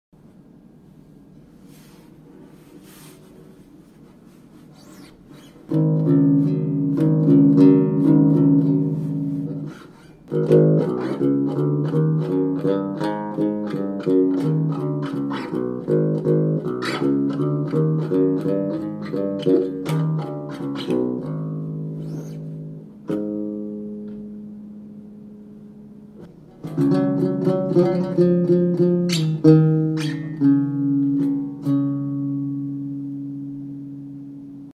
Colascione built and played by Jo Dusepo
I am no bassist, this is just to give an idea of the sound.